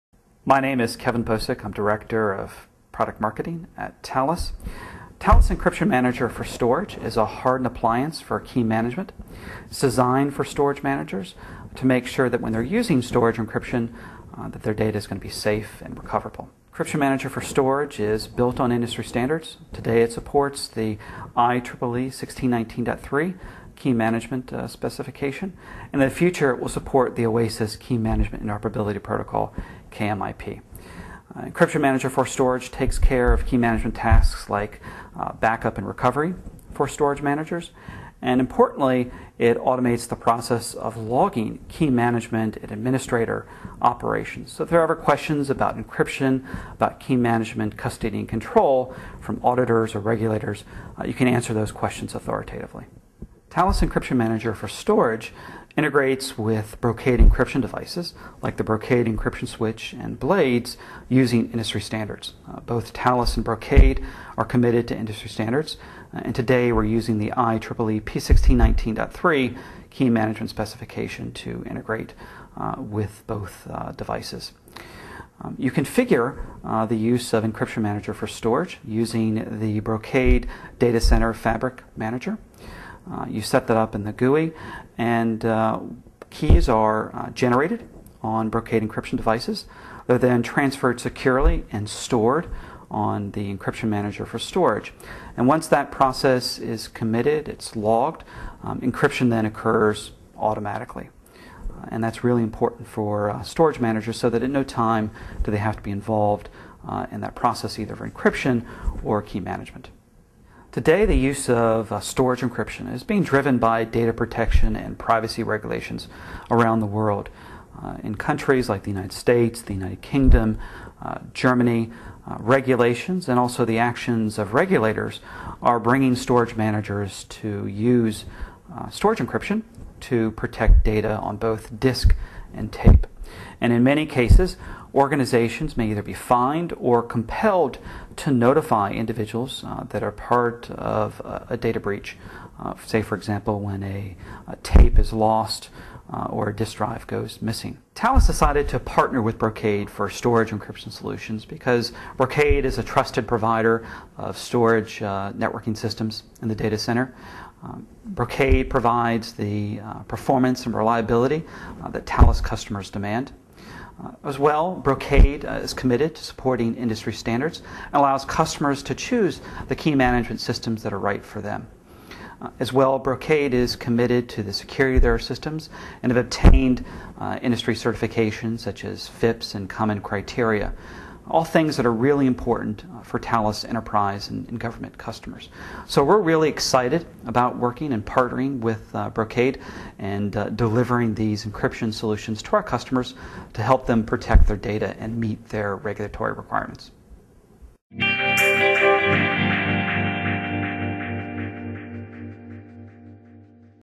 Brocade Encryption SAN Switching Solutions and Thales Encryption Manager
Kevin Bocek from Thales describes how combination of Thales Encryption Manager for Storage and the Brocade encryption SAN switching solutions helps protect enterprise data and helps allow organizations meet growing compliance demands.